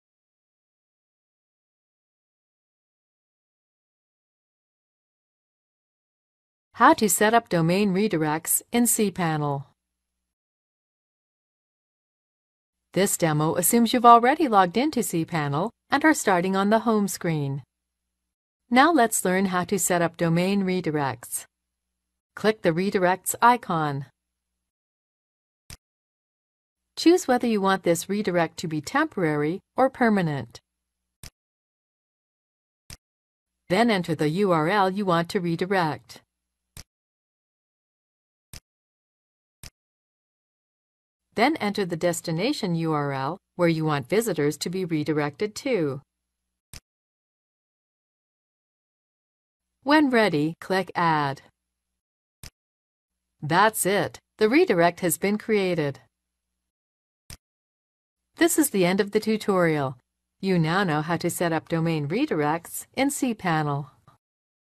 How to setup domain redirects in cPanel?
This demo assumes you've already logged in to cPanel, and are starting on the home screen.

Now let's learn how to setup domain redirects.

Click the Redirects icon.

Choose whether you want this redirect to be temporary or permanent.

Then enter the URL you want to redirect.

Then enter the destination URL where you want visitors to be redirected to.

When ready, click Add.

That's it! The redirect has been created.

This is the end of the tutorial. You now know how to setup domain redirects in cPanel.